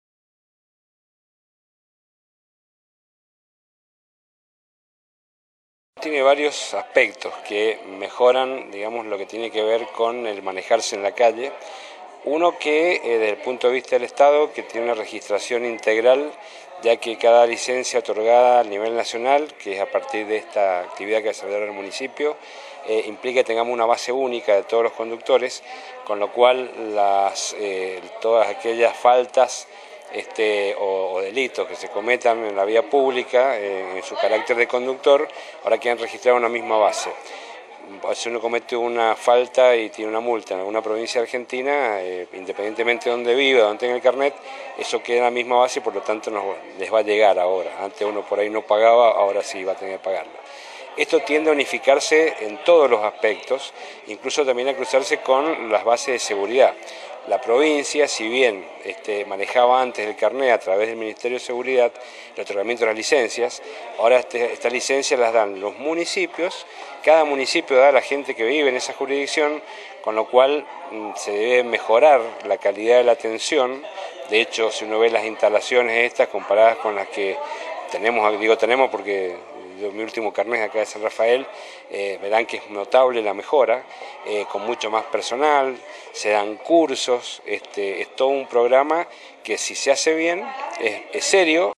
POR LA SEGURIDAD DE TODOS
La inauguración de un nuevo Centro de otorgamiento de licencias para conducir vehículos contempla información para la seguridad de todos los habitantes.